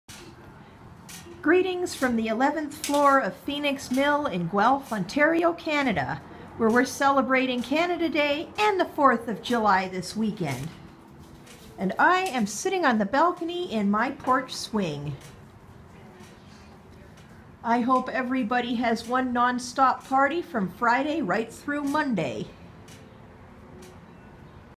Celebrating Canada Day & Independence Day in Canada
I'm a Canadian married to an American, so it's a bit of a tradition of ours to celebrate on the balcony